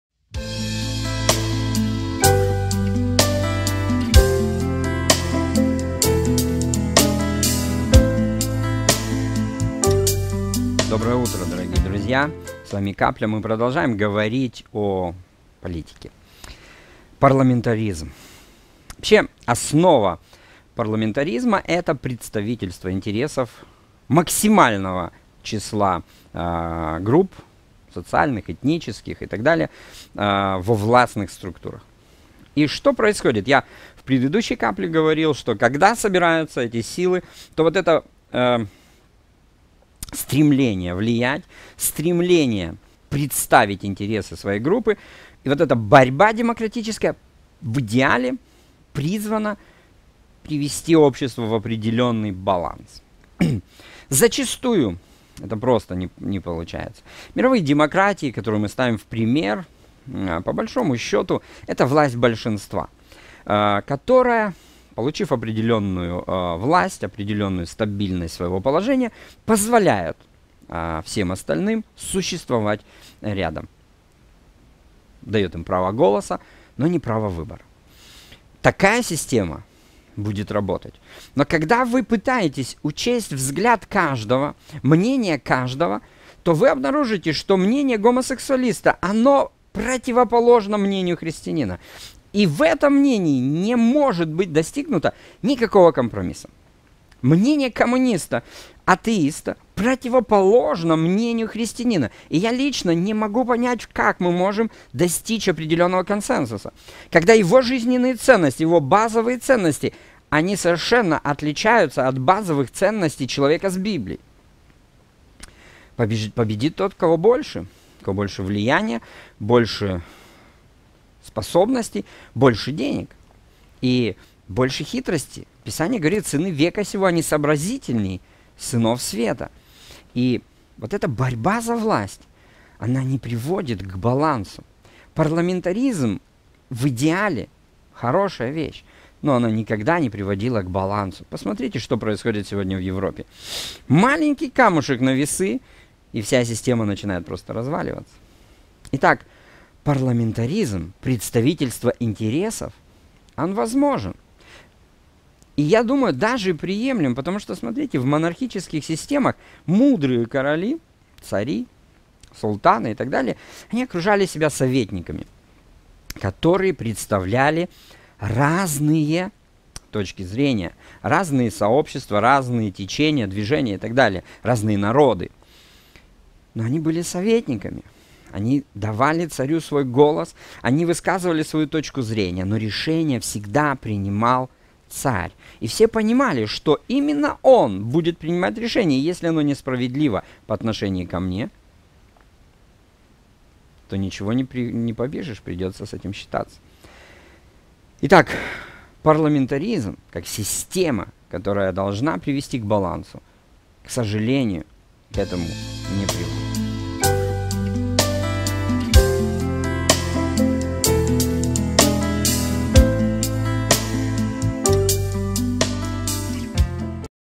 ПАРЛАМЕНТАРИЗМ
Это политическая система строящаяся на принципе представительства при которой голос каждой социальной группы будет услышан и надлежащим образом представлен.